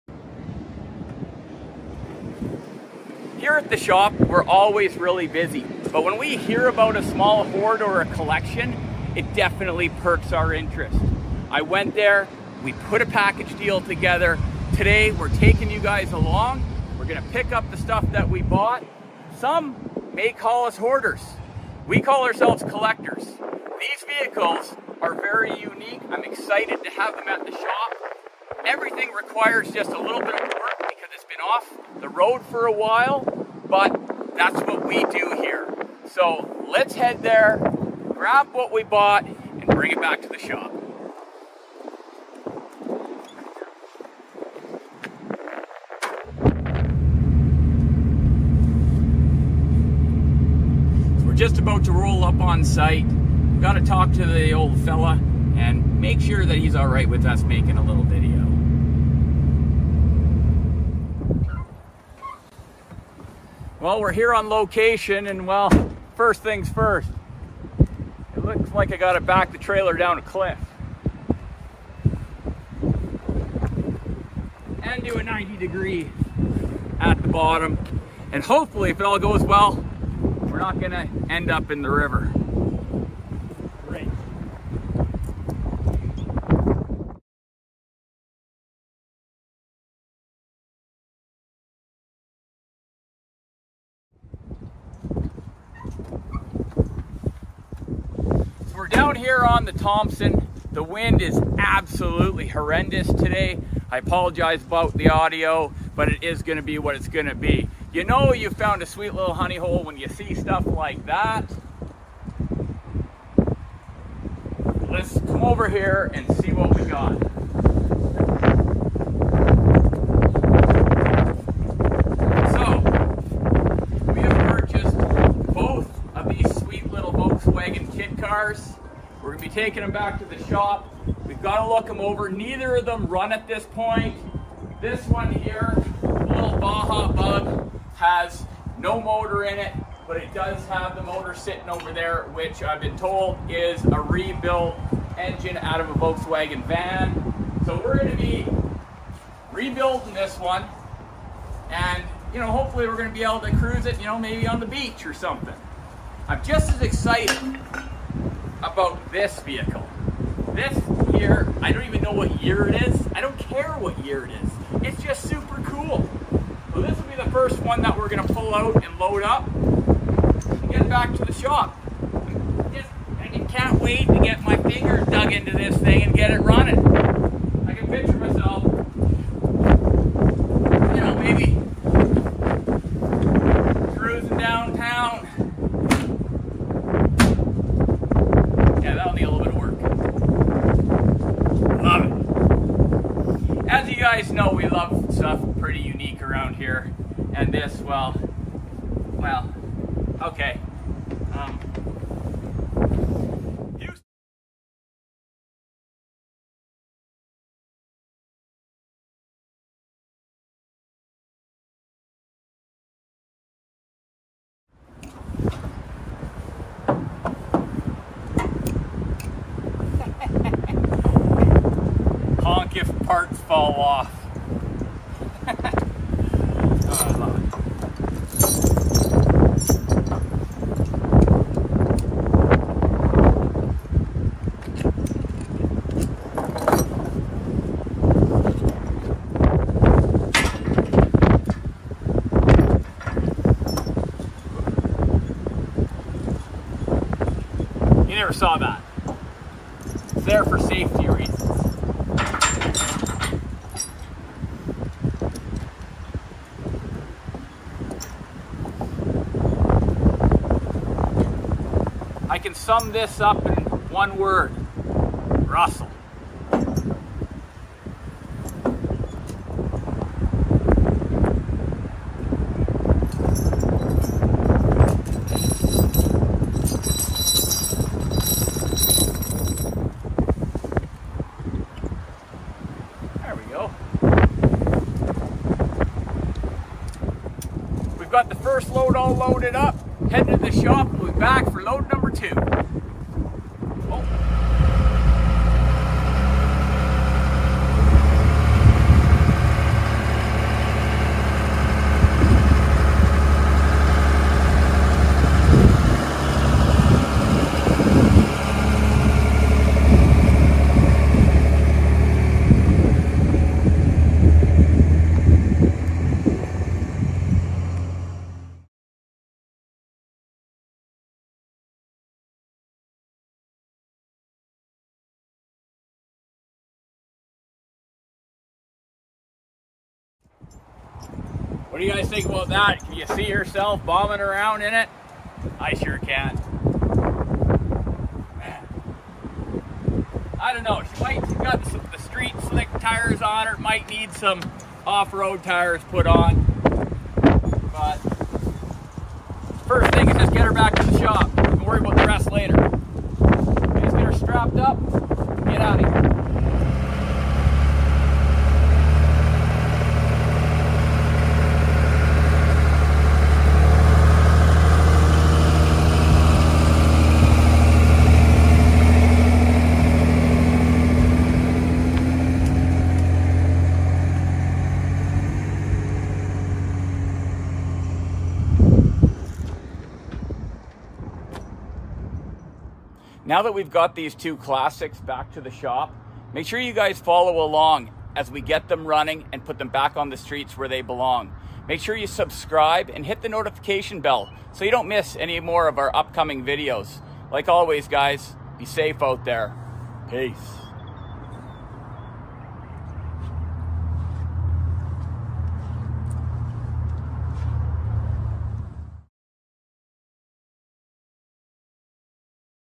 Just got a call about a Honey Hole! Get in and buckle up.
No time to waste. Lets go see what's there and make a deal!

MAILING ADDRESS:
Dubs Kustoms
1271-b Salish Rd
Kamloops, BC
V2H 1P6